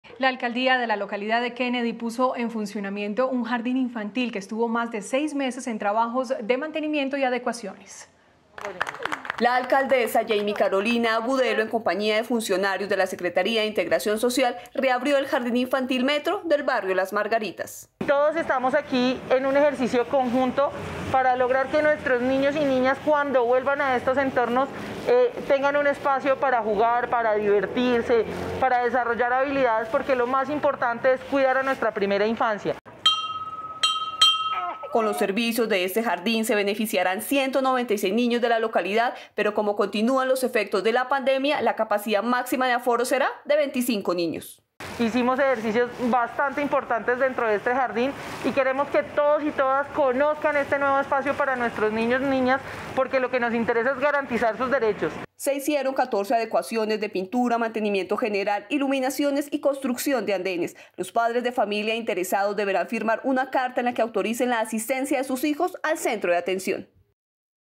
Reabren jardín infantil Metro en la localidad de Kennedy
NoticiasCapital | La Alcaldía de la localidad de Kennedy puso en funcionamiento un jardín infantil que estuvo más de seis meses en trabajo de mantenimiento.